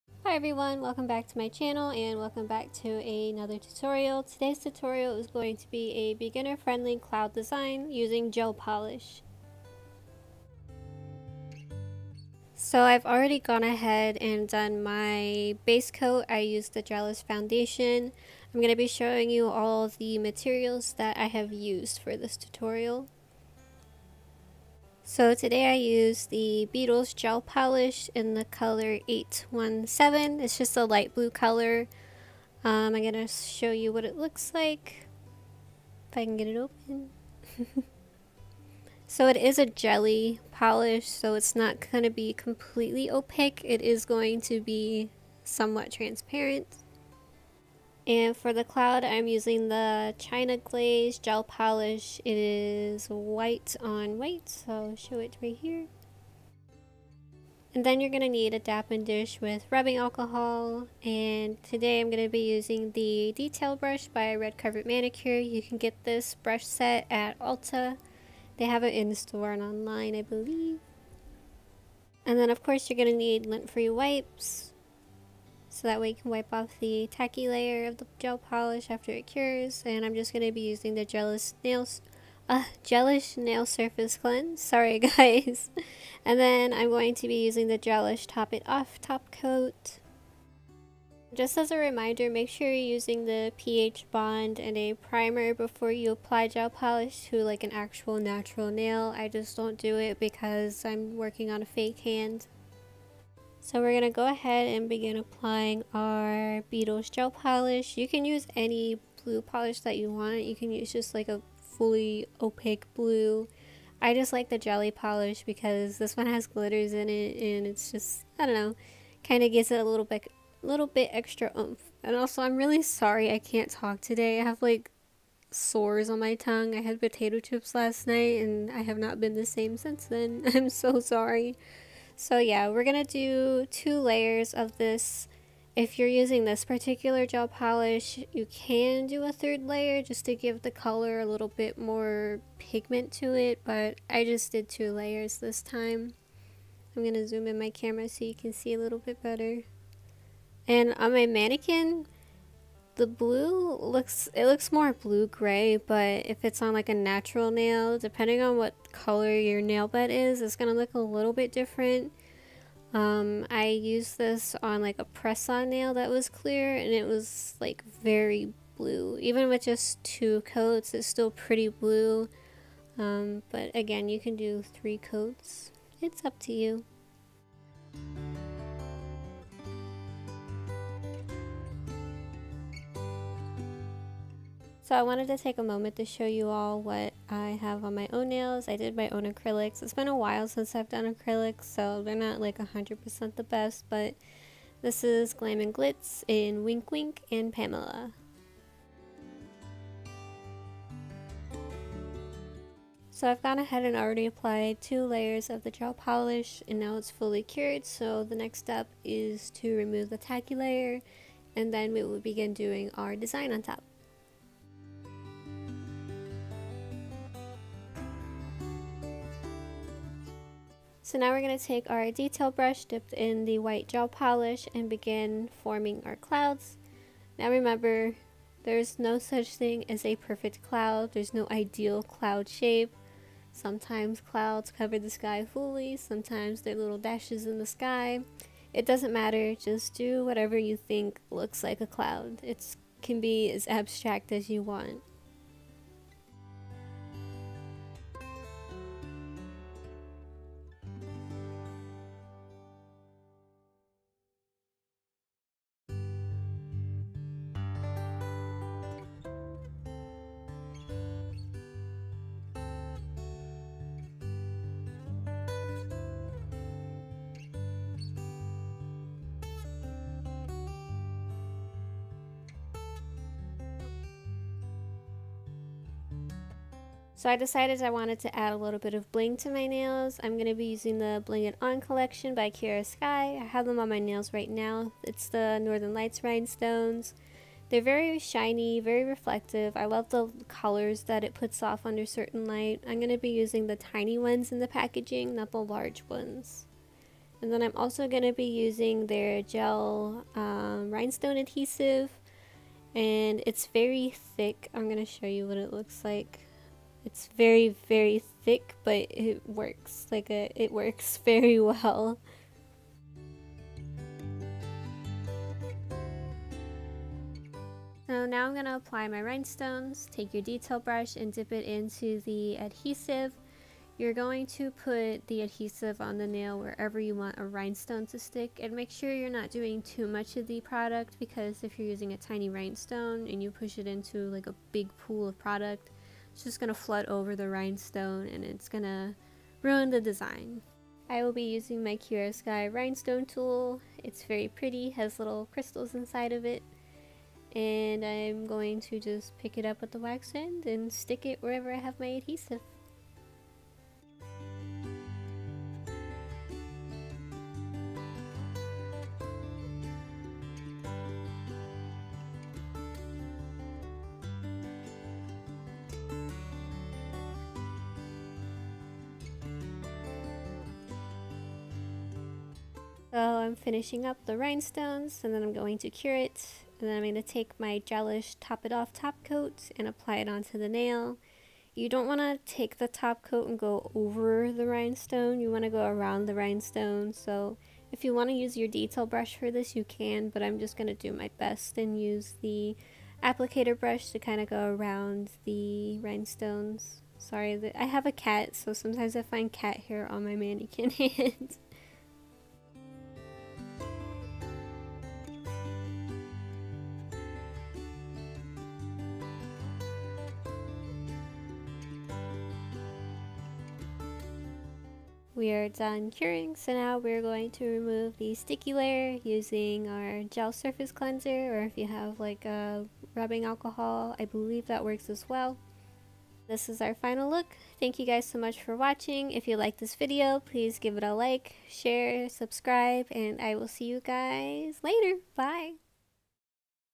Cloud Nail Design Tutorial | BEGINNER FRIENDLY | GEL POLISH
Hi everyone! Today I am showing you a sky/cloud nail design using gel polish!